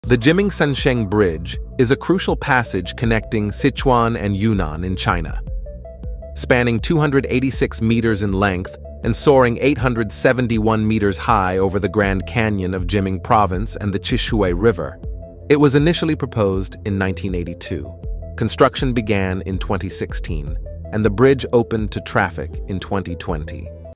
Bridge Construction in China.
The Jimingsansheng Bridge is a crucial passage connecting Sichuan and Yunnan in China. Spanning 286 meters in length and soaring 871 meters high over the Grand Canyon of Jiming Province and the Chishui River, it was initially proposed in 1982. Construction began in 2016, and the bridge opened to traffic in 2020.